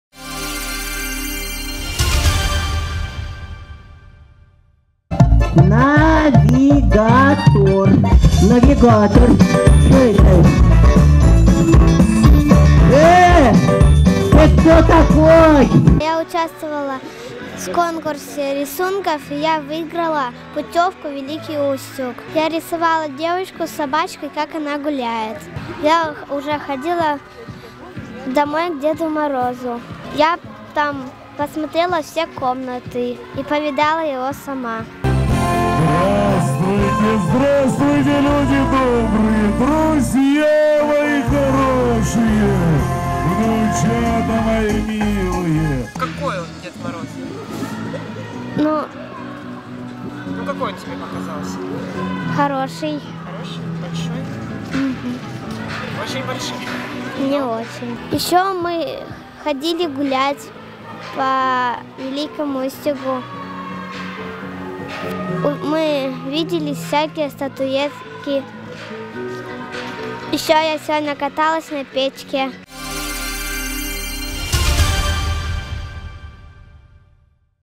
Победители конкурса рисунков «С днем рождения, Вологодчина» побывали на Вотчине Деда Мороза
Конкурс «С днем рождения, Вологодчина» проходил с июля по сентябре по инициативе Губернатора области и был посвящен 80-летнему юбилею региона. Участие в нем приняли больше 300 детей из разных уголков Вологодчины и других регионов. По итогам, победители конкурса, занявшие первые места, в качестве приза получили сертификаты, дающие право бесплатно посетить Вотчину Деда Мороза в Великом Устюге в День рождения зимнего волшебника.